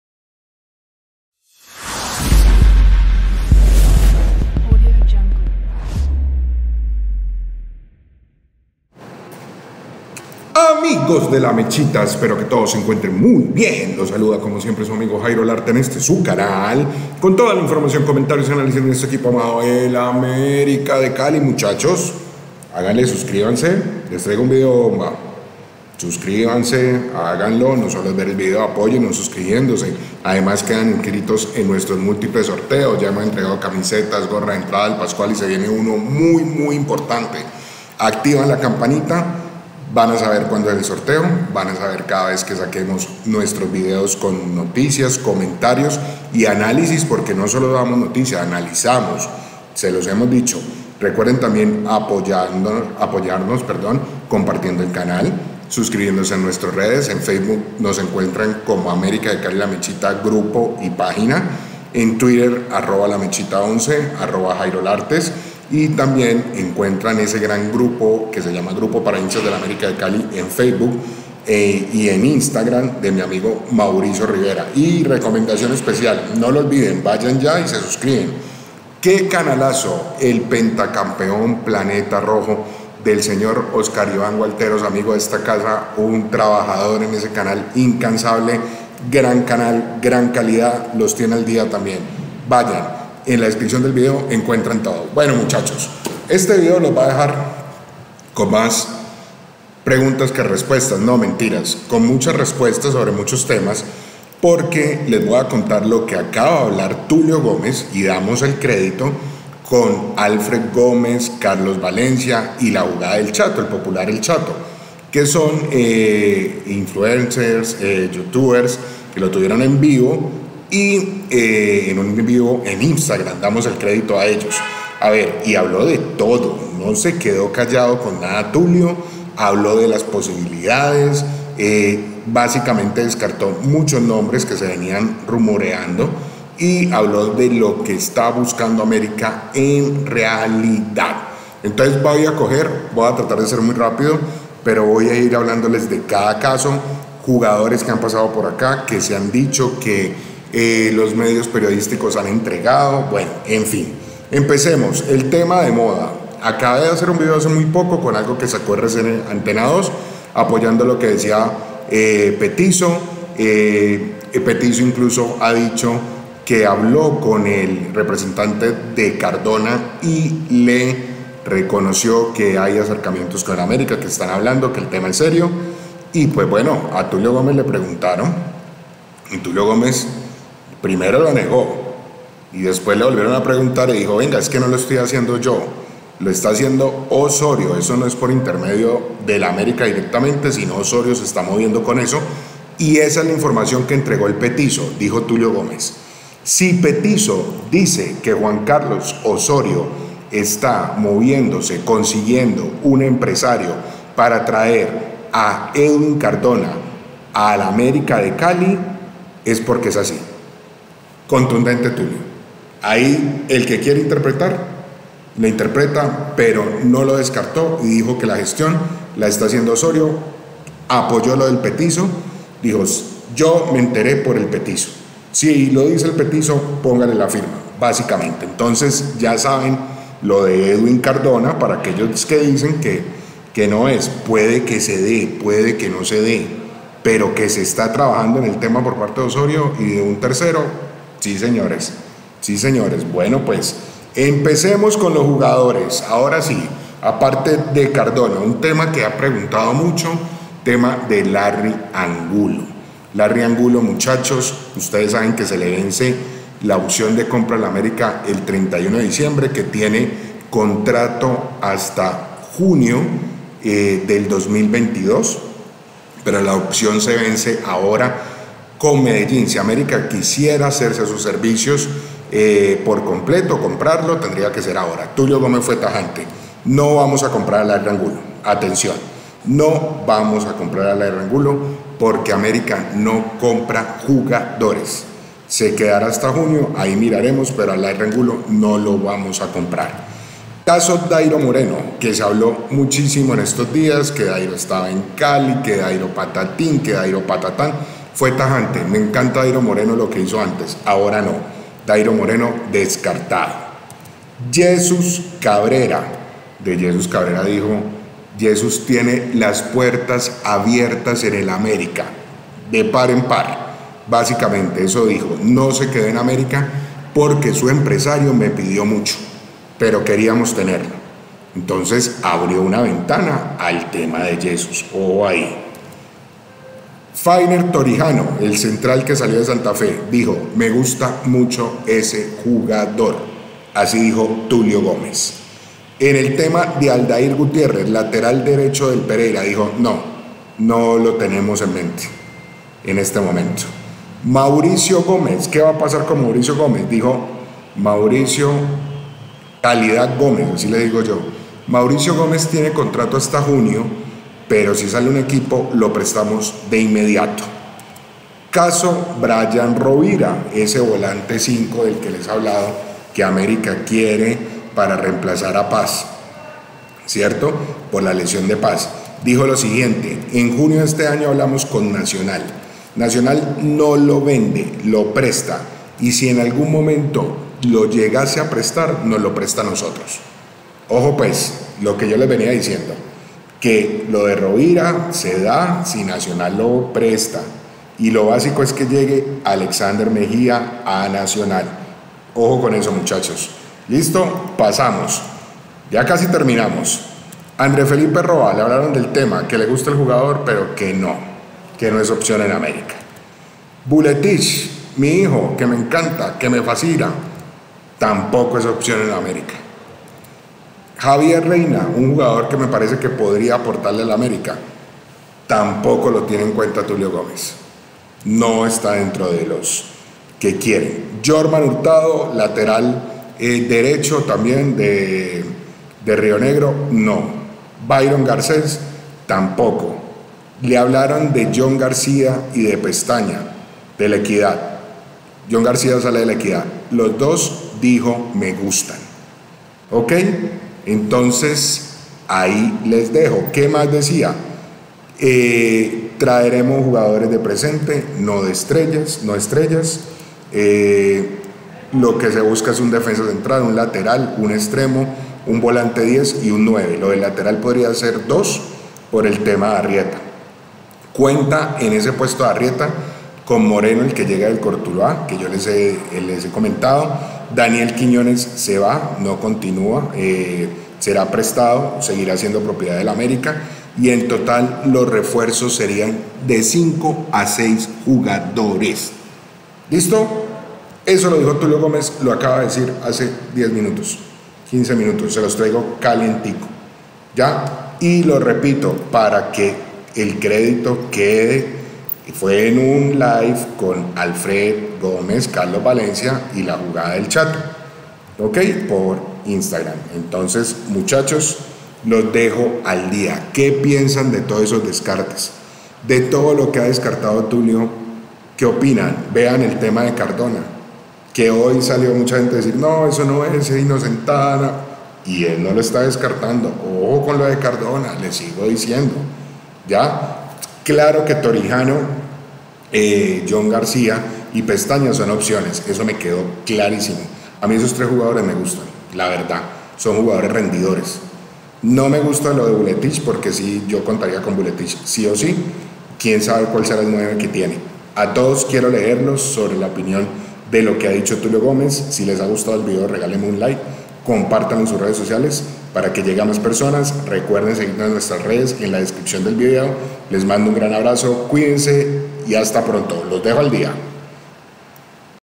💥 BOMBAZO🚨TULIO ACLARA TODO SOBRE CARDONA, LARRY ANGULO, DAYRO, YESUS,ROA Y ROVIRA ENTRE OTROS 😱
💥 ATENCIÒN!!!🚨 Tulio no se guardo nada y descarto y entregó las opciones de jugadores para el América en el 2022. Todo en @LaMechita